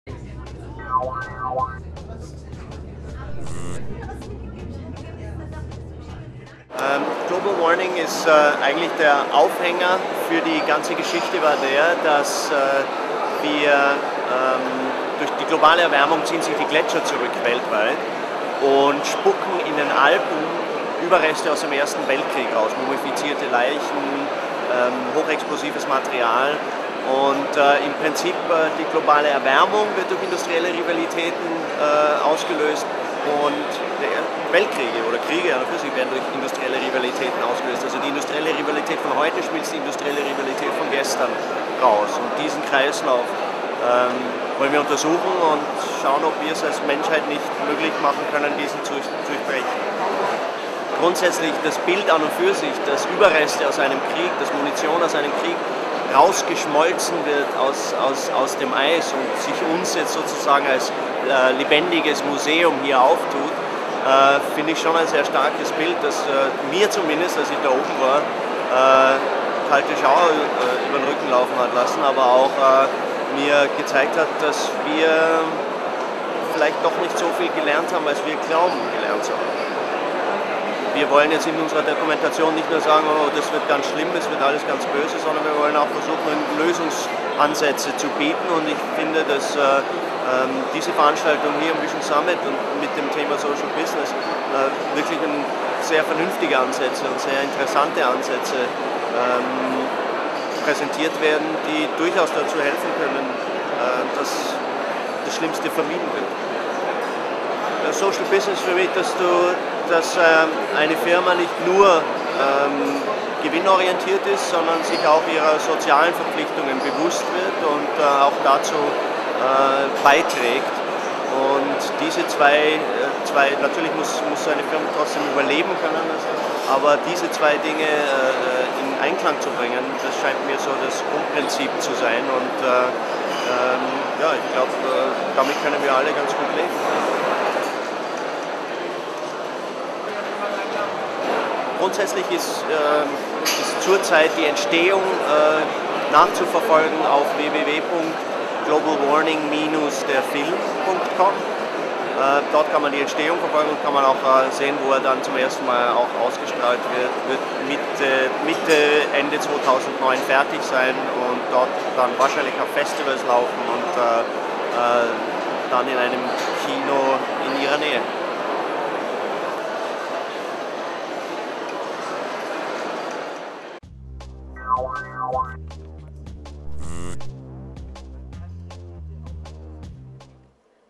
Global Warning Ernst Gossner
- Auf dem vision summit 2008 hat lebensunternehmer-tv den Regisseur Ernst Gossner zu seinem neuen Film "Global Warning" interviewt. Worum er geht, wann er rauskommt und was er persönlich von social business denkt.